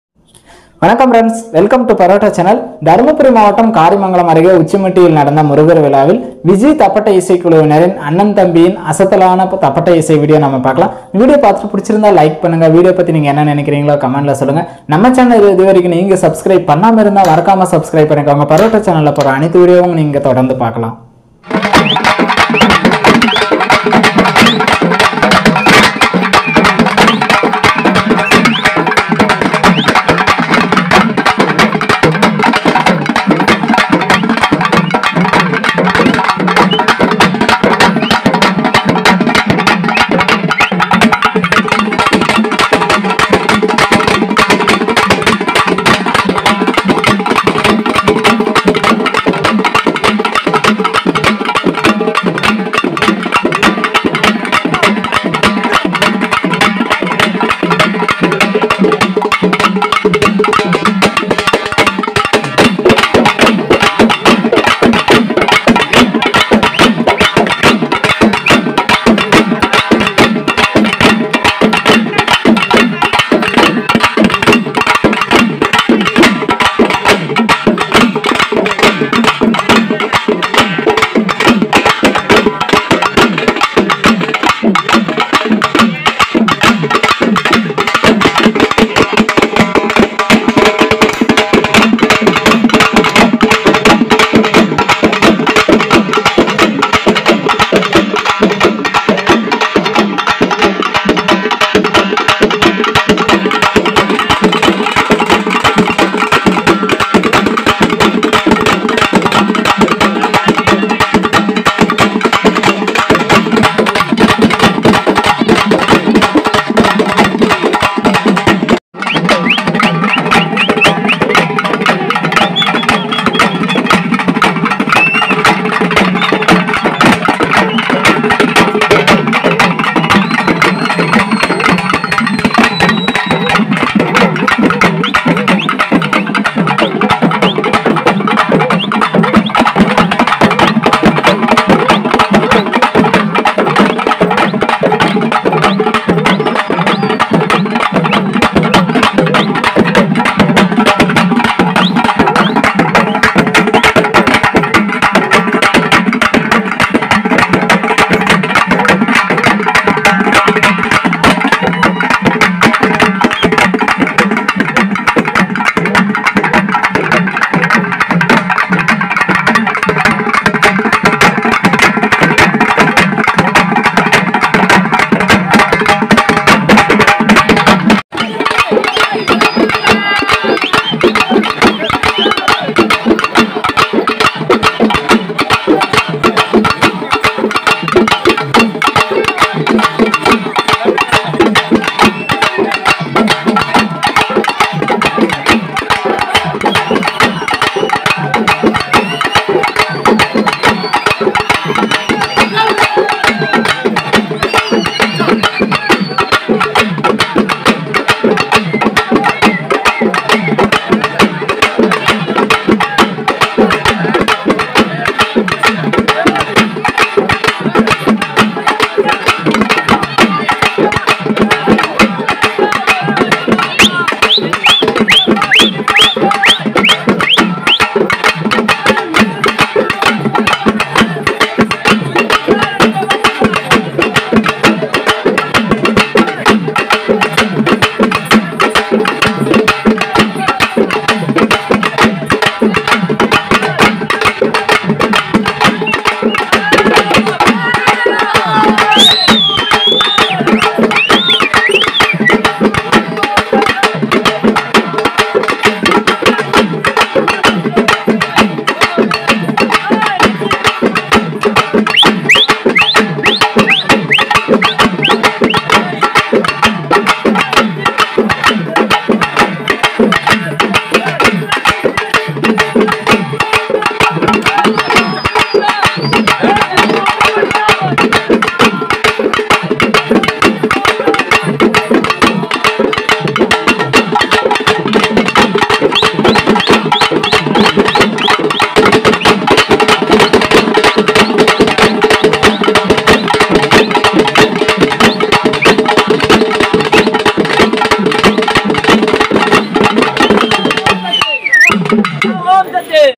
அண்ணன் தம்பியின் அசத்தலான தப்பட்டை இசை வாசிப்பு | Thappattai Music | Parotta Channel.
விஜி கிராமிய ஒத்தடி மேளம்
கிருஷ்ணகிரி

இந்த வீடியோவில் அண்ணன் தம்பியின் அசத்தலான தப்பட்டை இசை வாசிக்கும் வீடியோ பார்க்கலாம்.